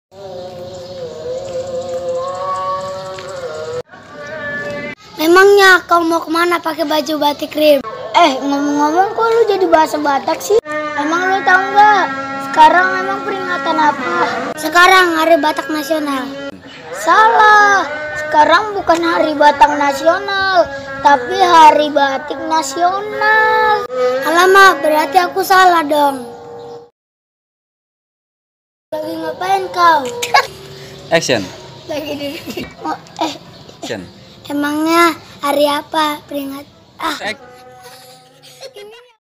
Film pendek !! HARI PERINGATAN BATIK NASIONAL INDONESIA !!
Film arim adalah konten hiburan untuk anak-anak Arim menggunakan baju batik menunjukan bahwa dia cinta tanah air indonesia,peringatan hari batik nasional bertujuan untuk menghormati budaya dan peninggalan nenek moyang kita